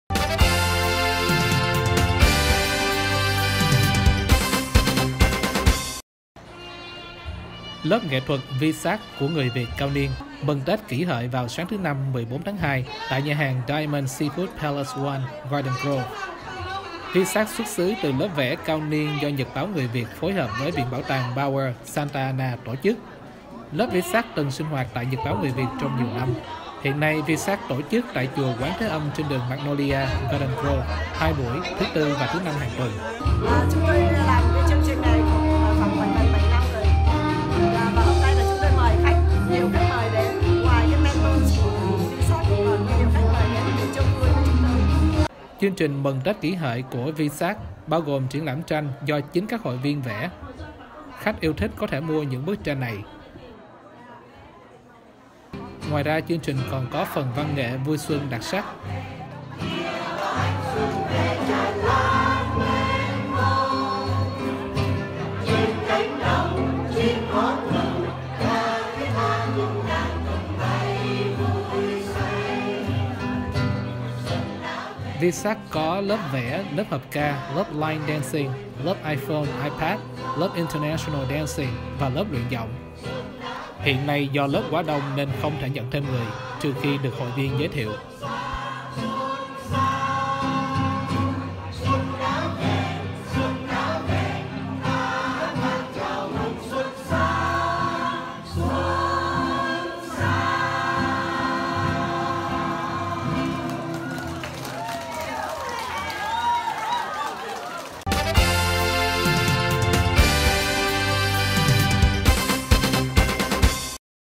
Lớp Nghệ Thuật VSAC mừng Tết Kỷ Hợi
Lớp Nghệ Thuật VSAC của người Việt cao niên mừng Tết Kỷ Hợi vào sáng Thứ Năm, 14 Tháng Hai, tại nhà hàng Diamond Seafood Palace 1, Garden Grove.